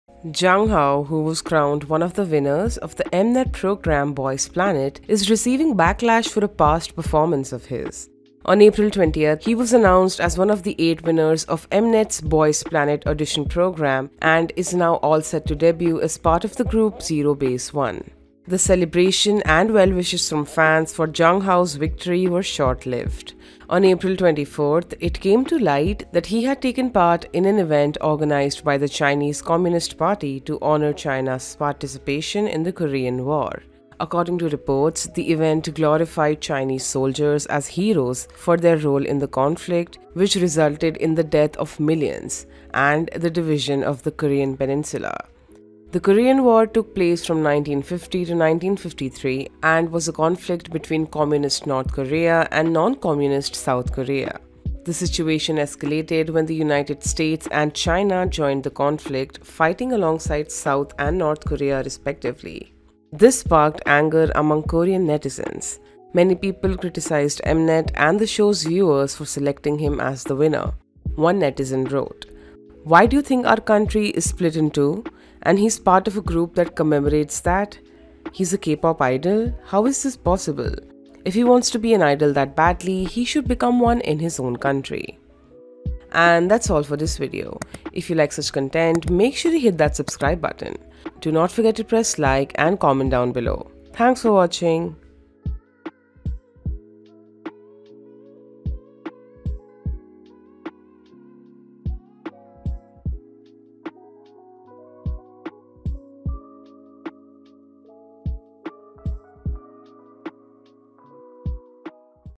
Boys Planet Zhang Hao Is Under Fire After Past Discovered
Zhang Hao, who was crowned one of the winners of the Mnet program Boys Planet, is receiving backlash for a past performance of his. On April 20, he was announced as one of the 8 winner of Mnet‘s Boys Planet audition program and is now all set to debut as part of the group ZEROBASEONE.
The celebration and well-wishes from fans for Zhang Hao's victory were short-lived as it was discovered that he had taken part in a an event organized by the Chinese Communist Party in the past.